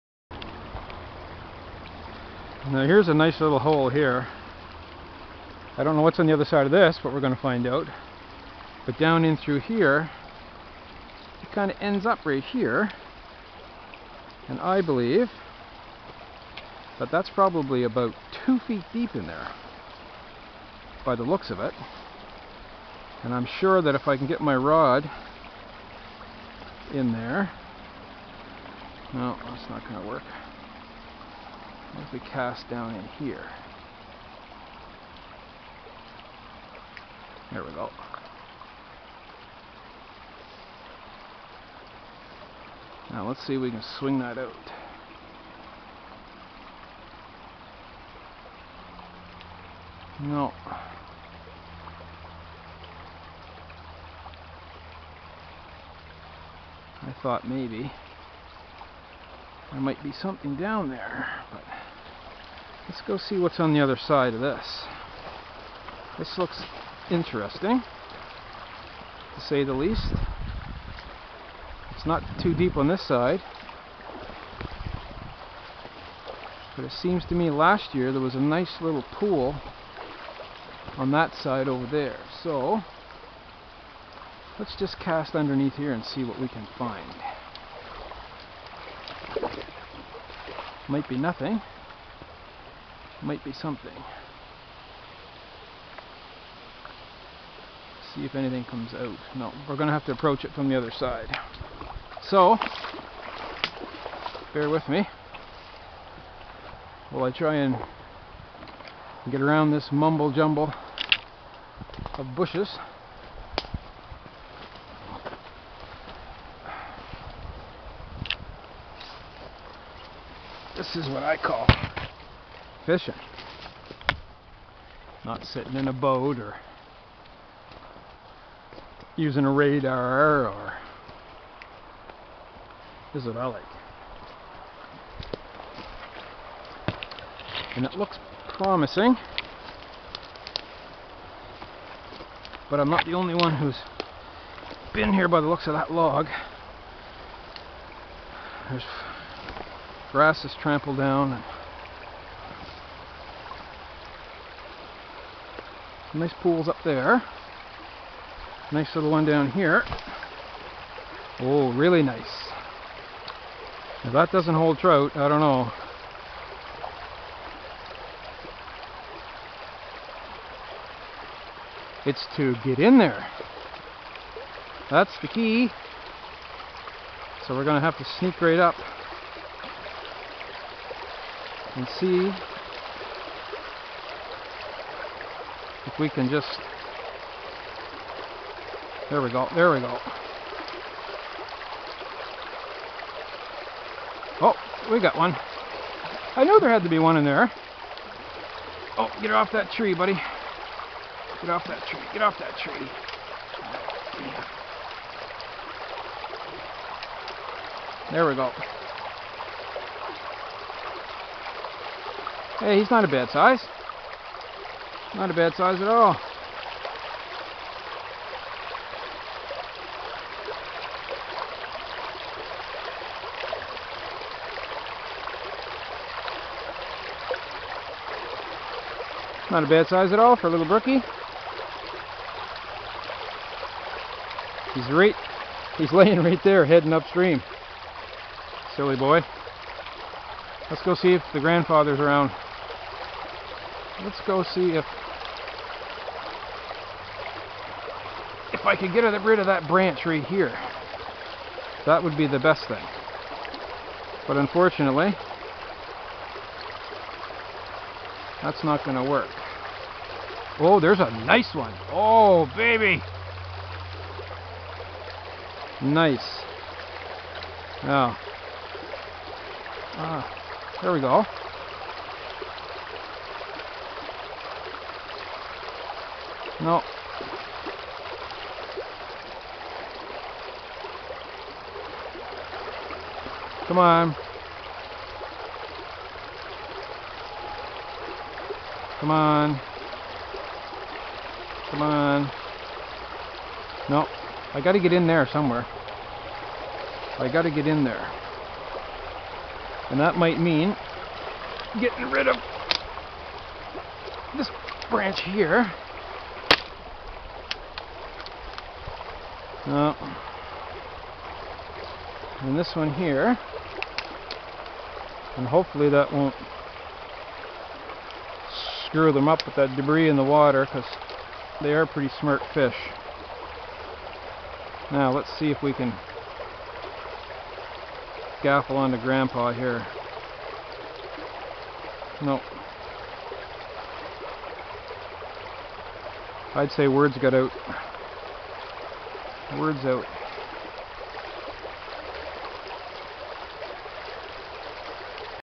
Sussex Brookie # 3
Short vid about brook trout fishing outside of Sussex, NB